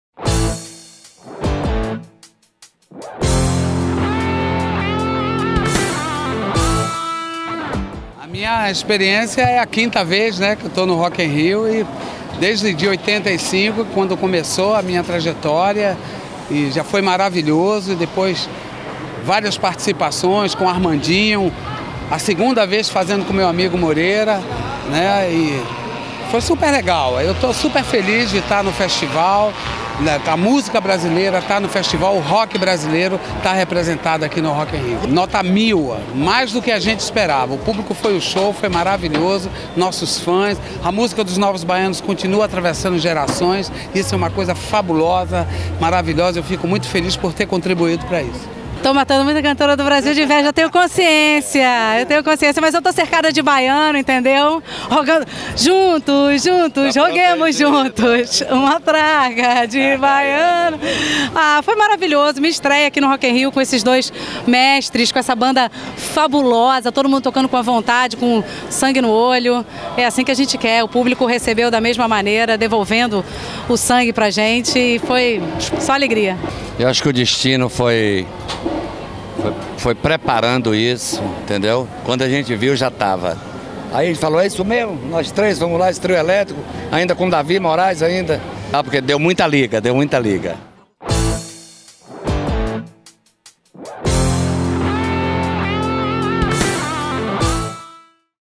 Rock in Rio entrevista: Moraes Moreira, Roberta Sá e Pepeu Gomes
Roberta Sá, Moraes e Pepeu resgataram a história dos Novos Baianos no Rock in Rio. Confira os bastidores dessa união de peso que deu muita liga no Palco Sunset do Rock in Rio.